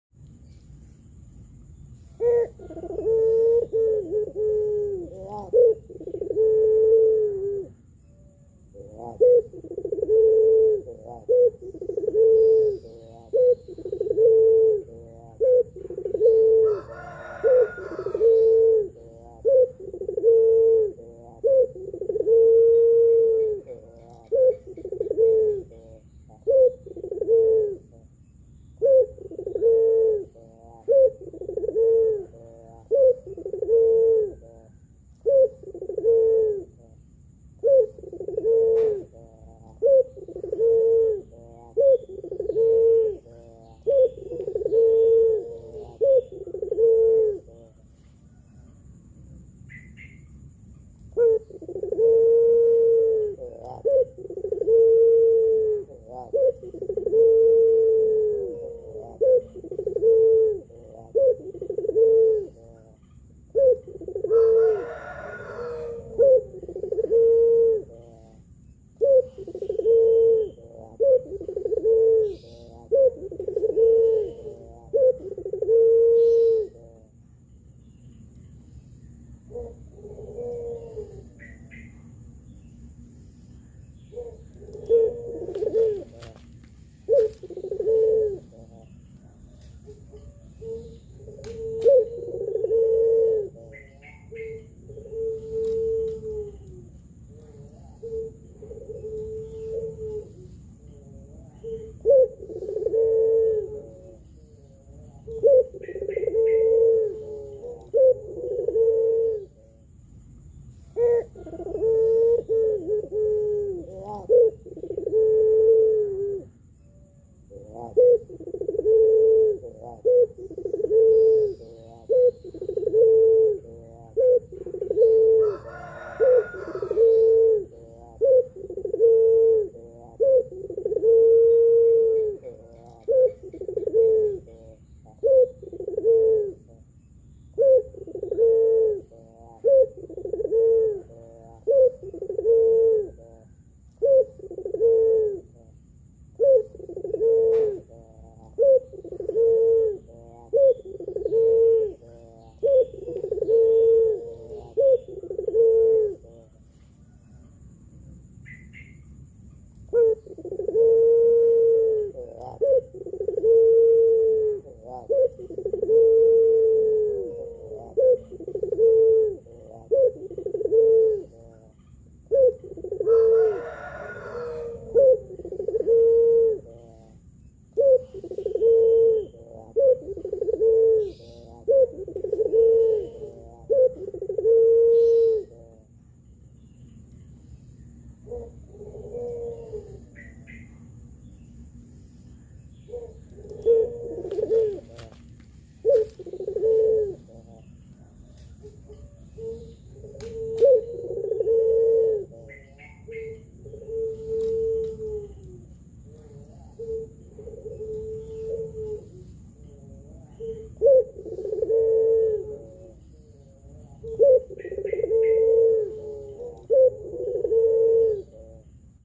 PUTER PELUNG RAJA PANCING COCOK BUAT PUTER BARU BELAJAR BUNYI
Puter pelung taja pancingan
Cocok buat pemancing puter puter malas bunyi ataupun yg baru belajat bunyi